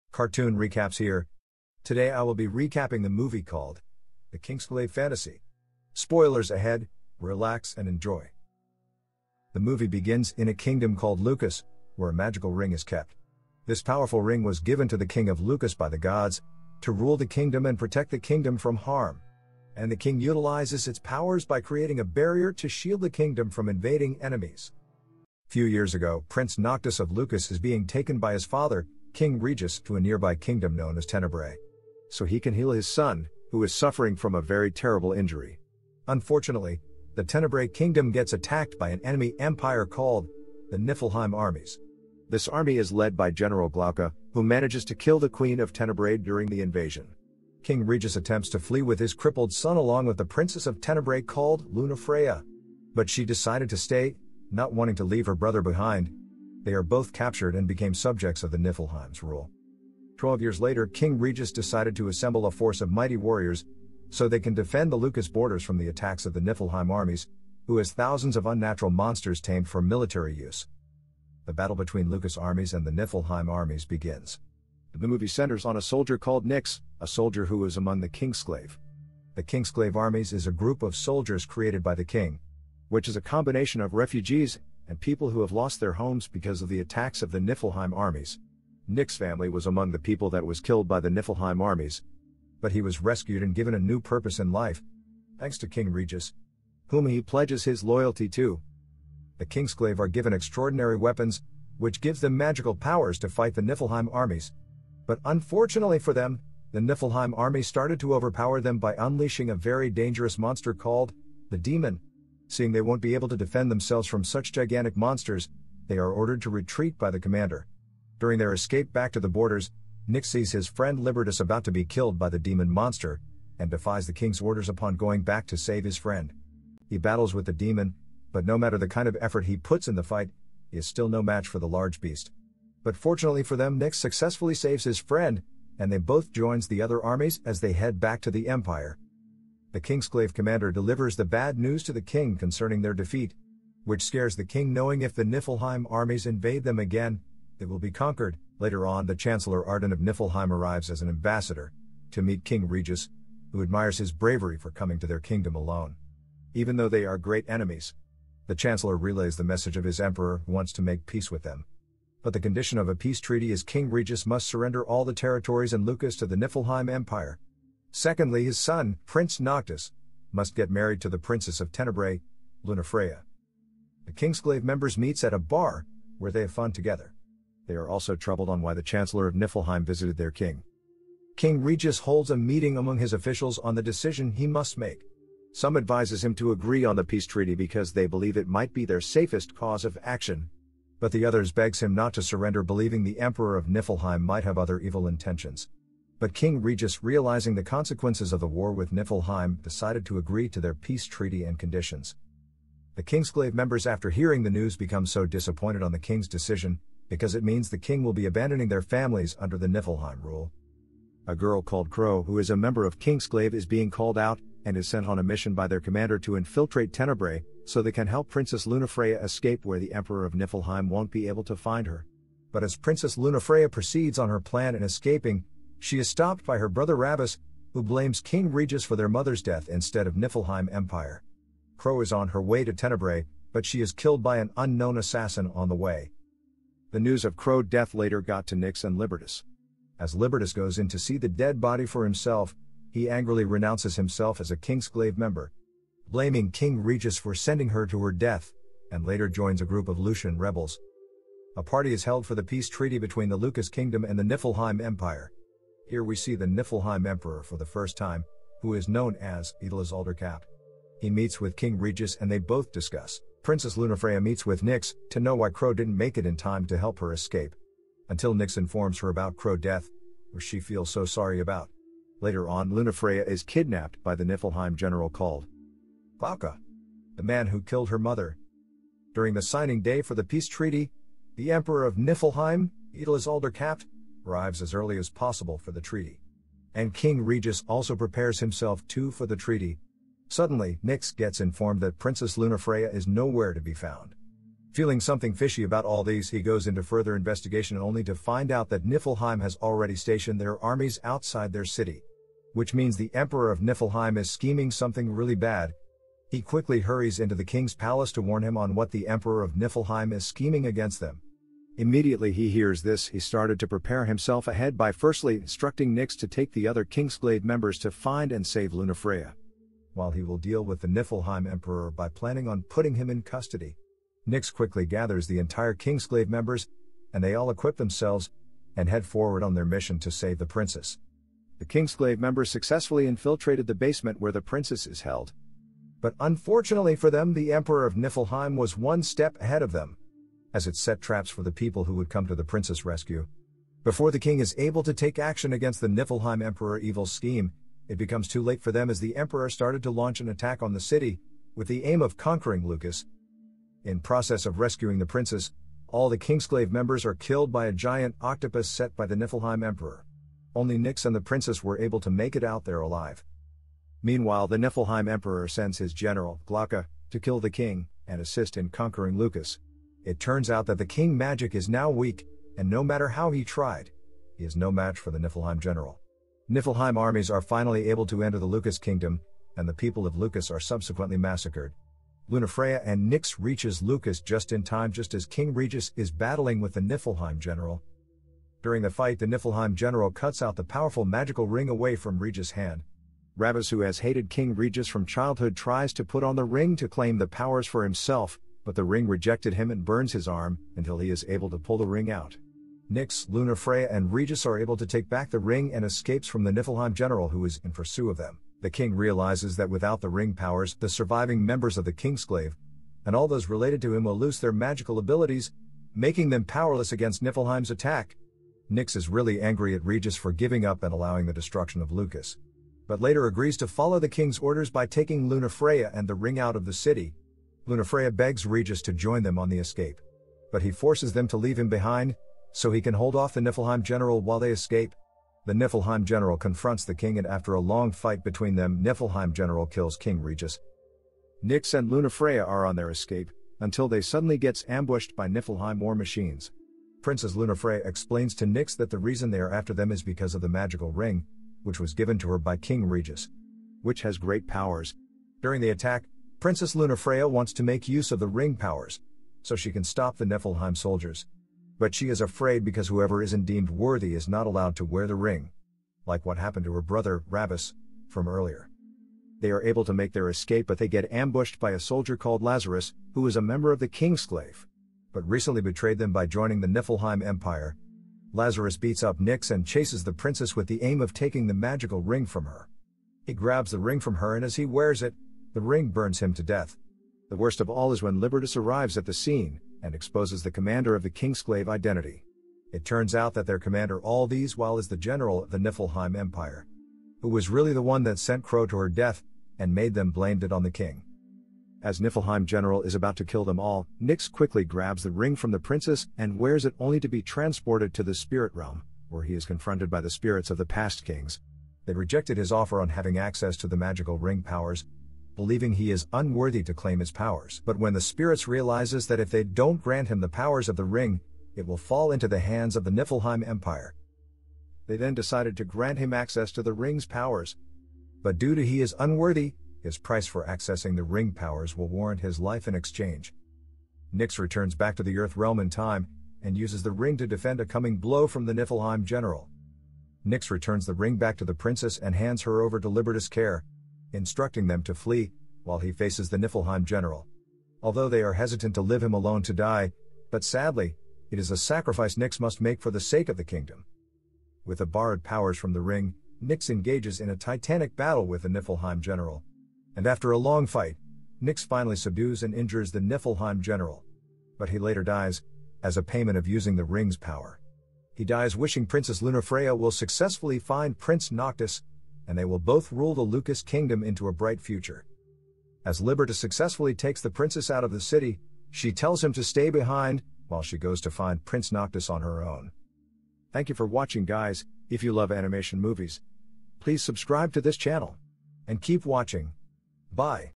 This King Is Given Powers By The Gods To Protect His Kingdom From Unnatural Monsters
This video talks about a king, who is given magical powers by the gods to protect his kingdom from harm.

anime recaps,anime recaps,sci fi movies,